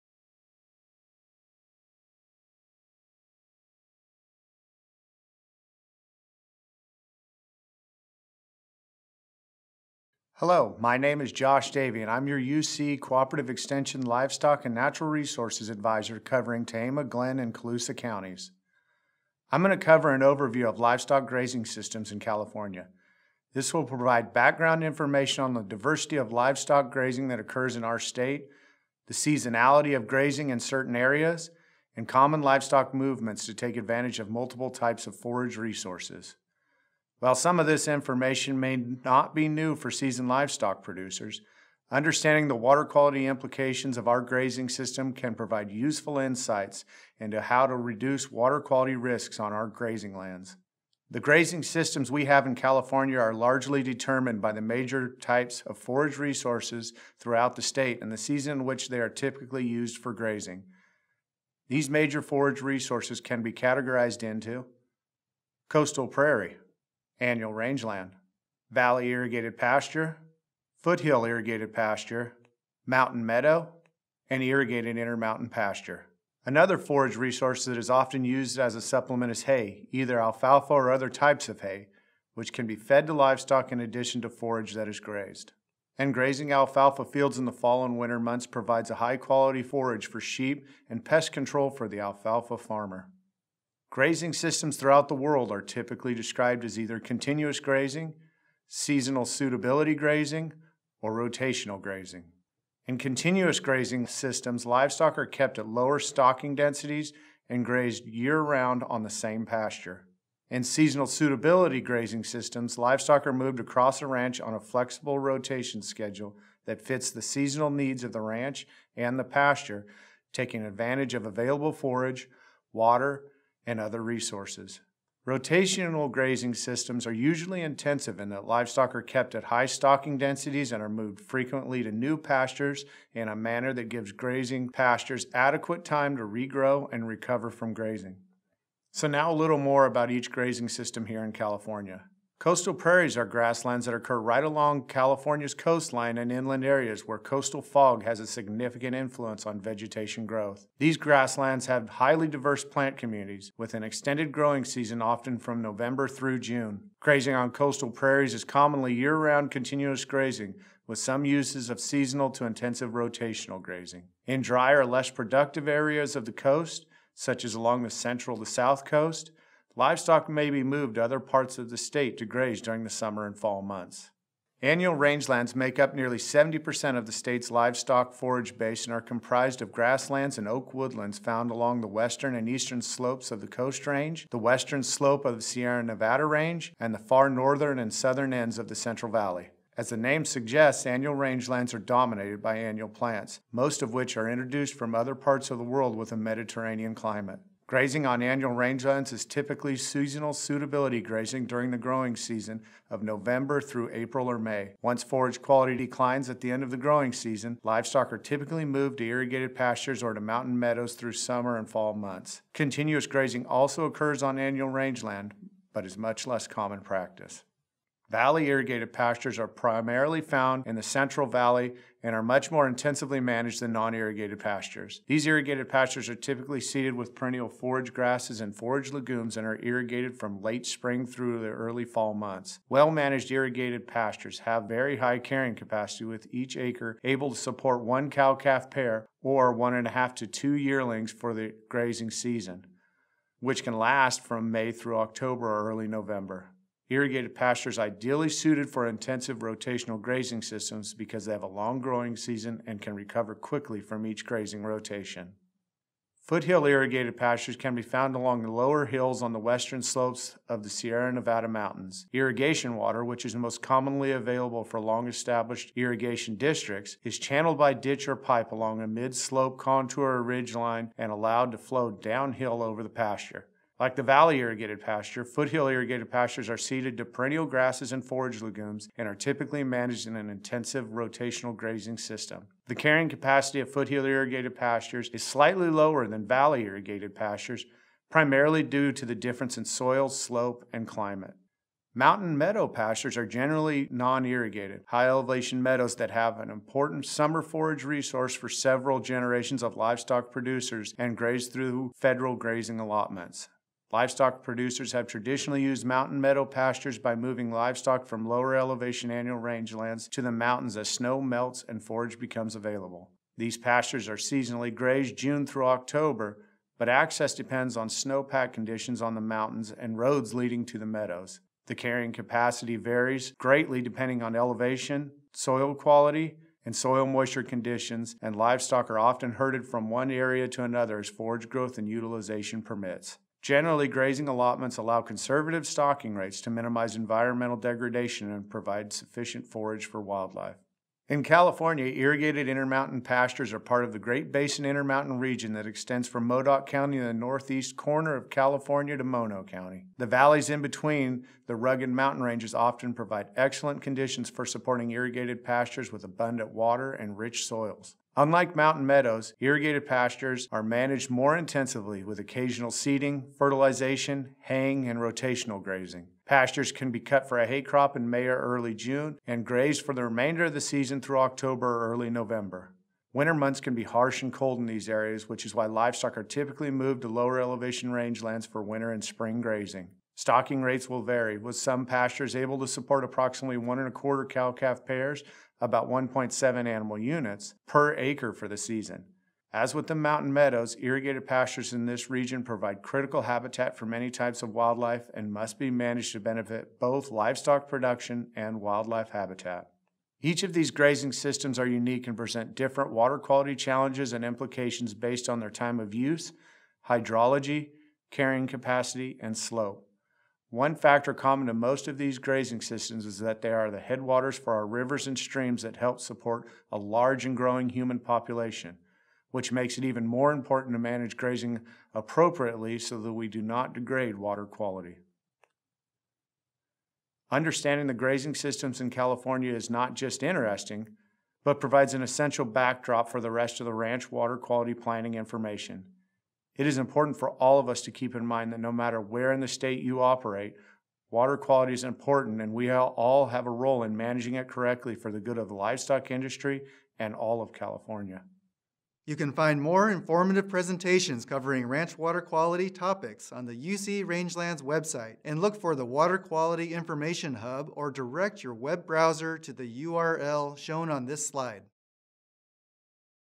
California Grazing Systems
Josh Davy describes California grazing systems based upon climate and vegetation differences across the state.